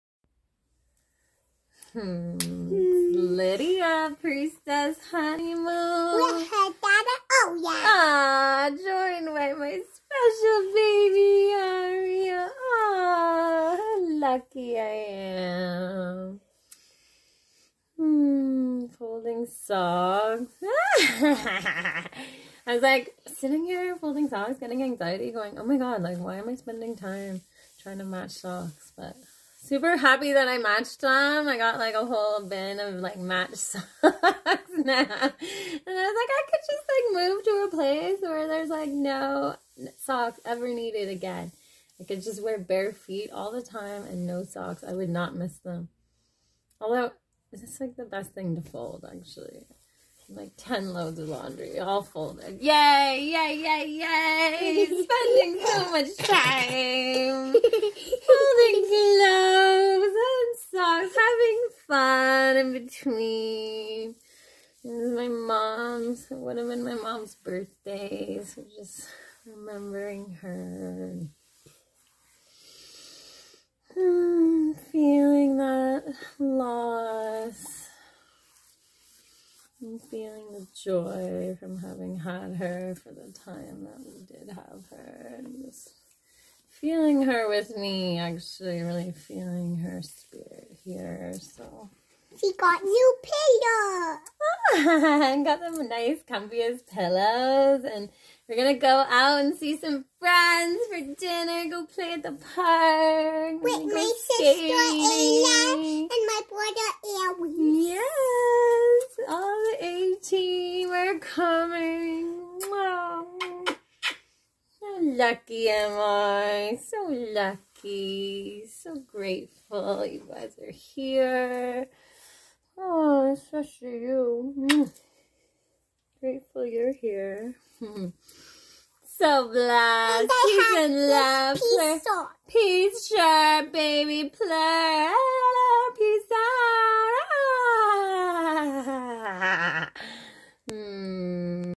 Day #57 365dayvlogchallenge round2 grief joy love being easyonme daughter proud mama
Having FUN with the bebes. Taking it easy on ME! Missing my Mom. Remembering and honouring her in every way I can. She is here with me. I am so LUCKY.

PLUR